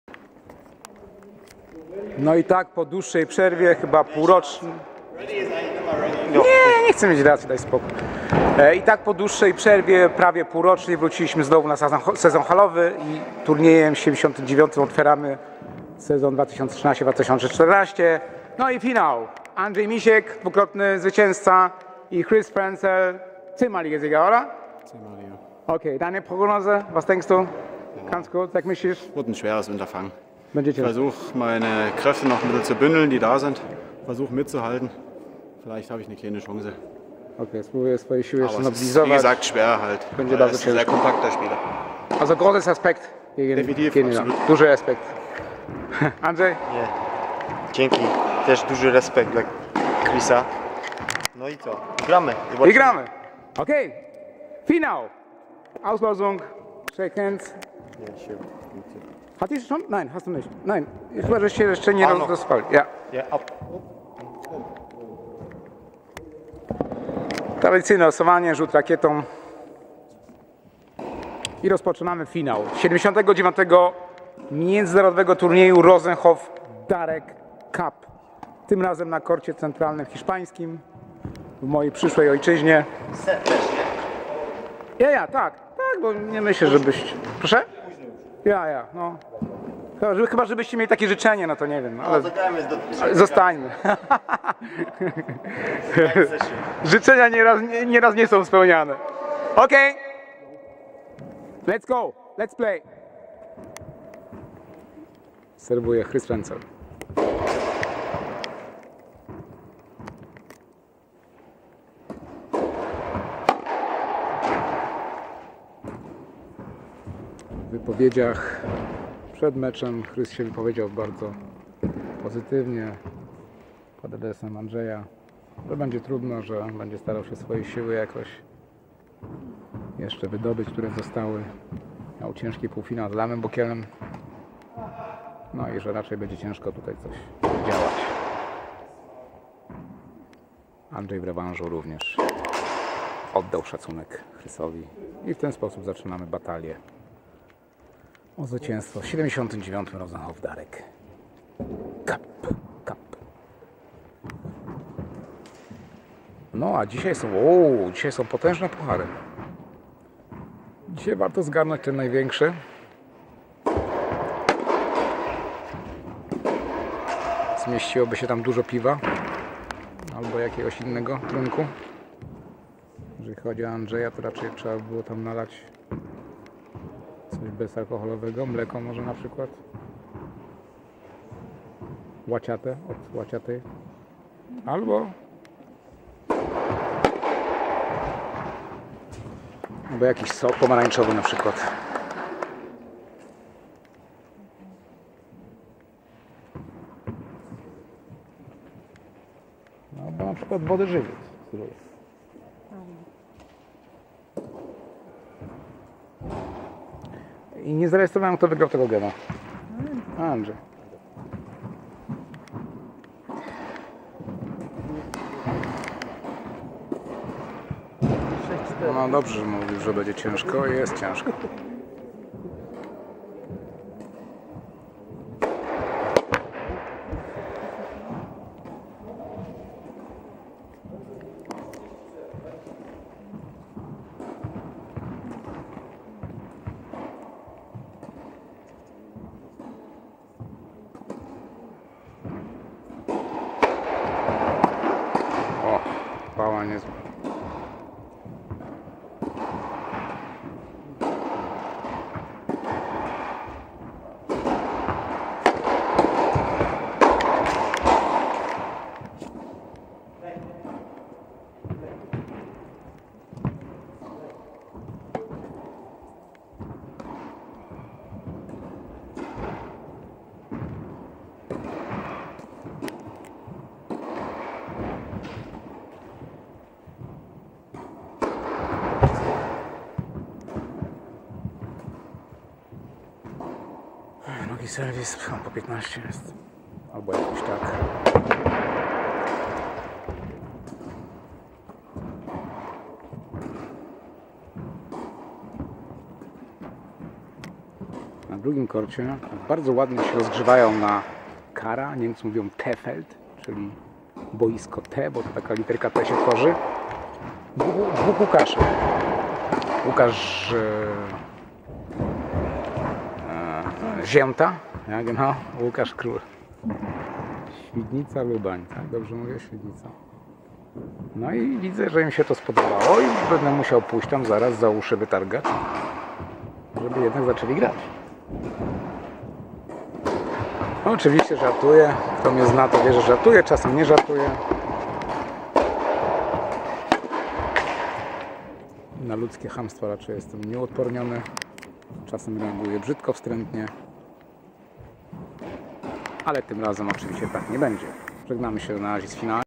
79 Rosenhof darek Cup.Main Tour.Final (p.1)A.MISIEK vs Ch.FRENZEL.
79 Cup, Görlitz 20.X.2013 International Tennis Tournament ROSENHOF darek CUP -  Main Tournament Final Match, part 1 - Andrzej MISIEK Świdnica vs Chris FRENZEL Görlitz  ***  Turniej główny, mecz finałowy, część 1.